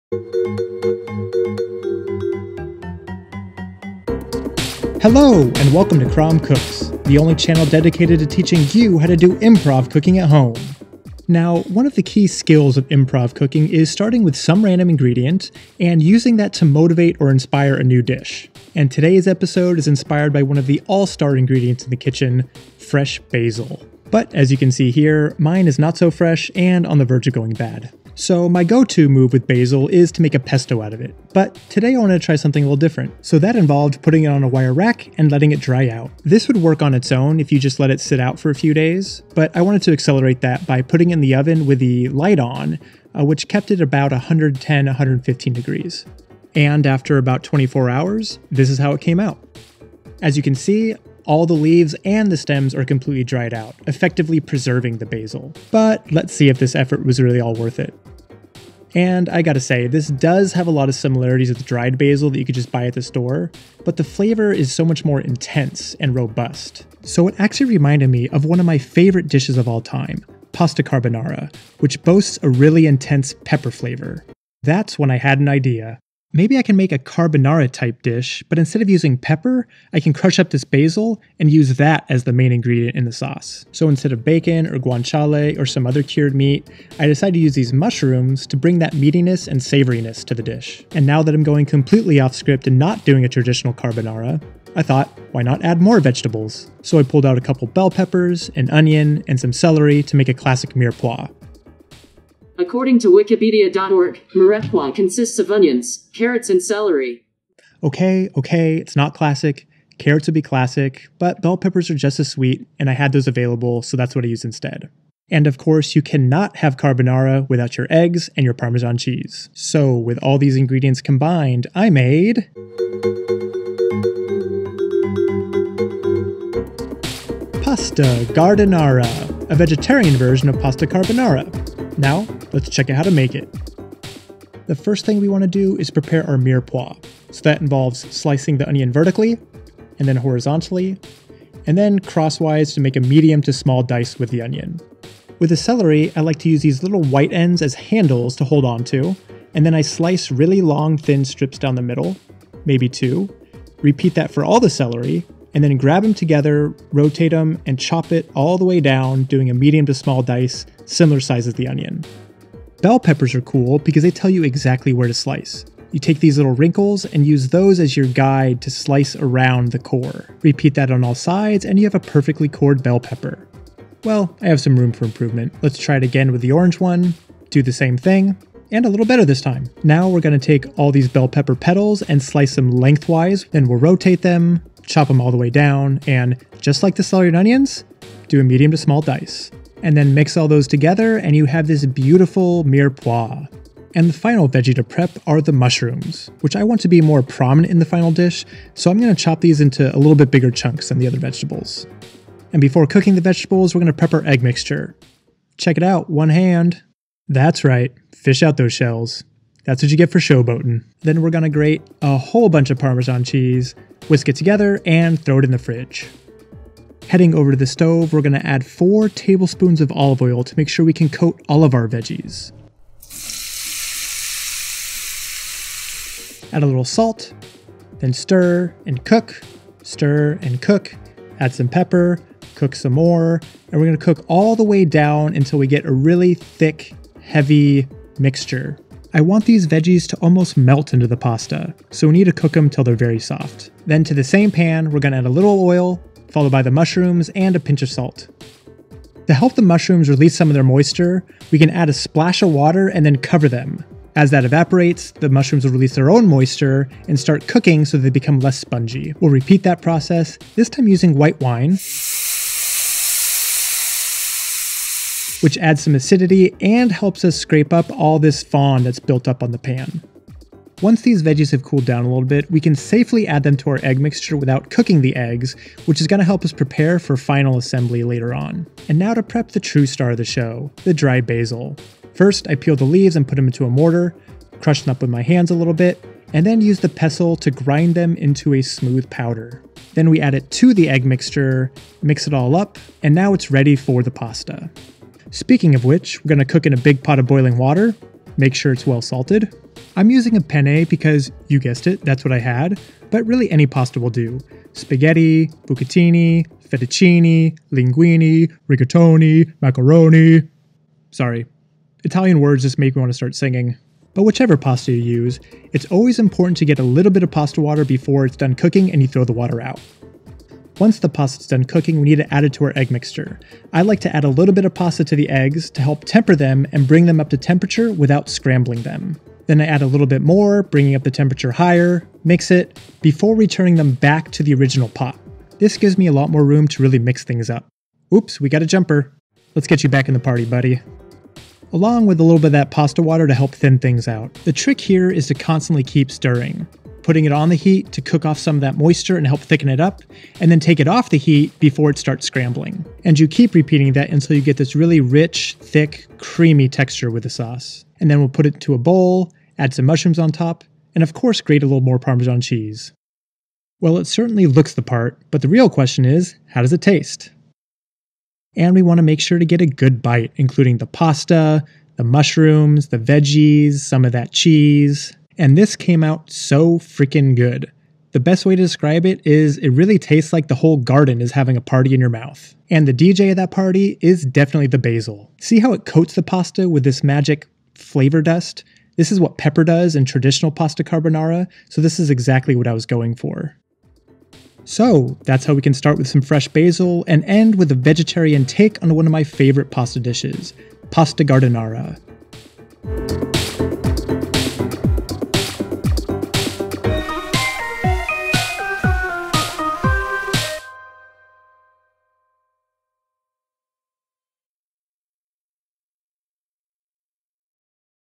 Improv Cooking with... Basil, Veggies & Eggs | Pasta Gardenara | CromCooks
This episode shows you how you can start with a single ingredient (fresh basil) and use it to inspire a completely new dish, in this case a vegetarian version of the classic, pasta carbonara! Hope you enjoy.

Name of dish: Pasta Gardenara